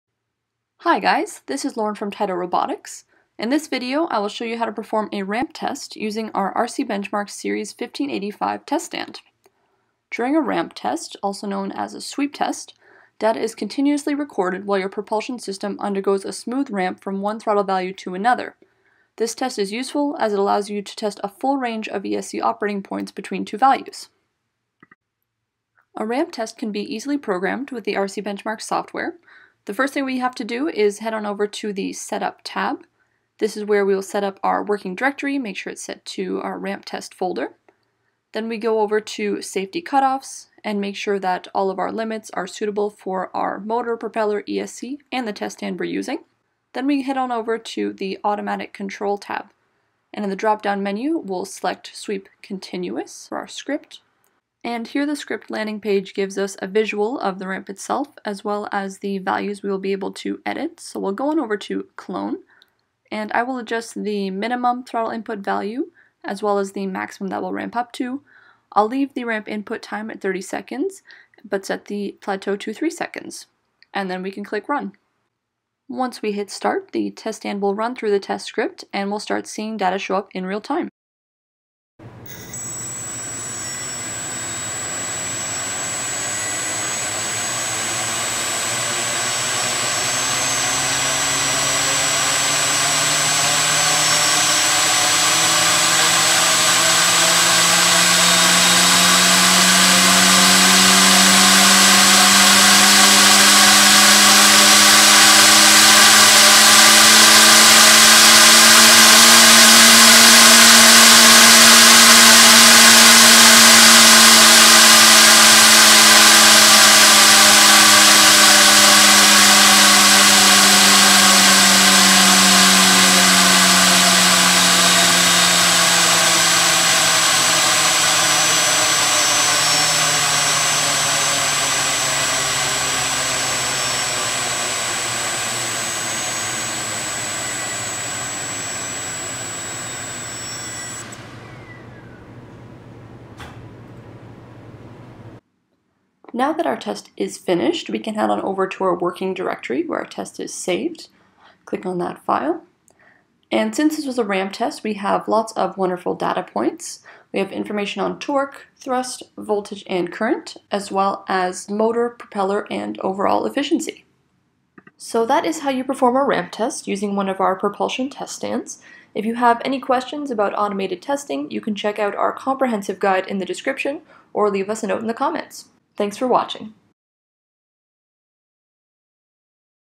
How to Perform a Ramp Test with Brushless Motor Thrust Stand
In this video we show you how to perform a propulsion ramp test (AKA a sweep test). We also discuss how the test is designed and why you would want to perform one. The demonstration is performed with our RCbenchmark Series 1585 test stand.

0:00 - 0:08 - Introduction
0:09 - 0:24 - What is a Ramp/Sweep Test
0:25 - 1:14 - Set-up Test in Software
1:15 - 2:24 - Run the Ramp Test
2:25 - 2:43 - Viewing the Data
2:44 - 2:59 - Conclusion + Testing Guide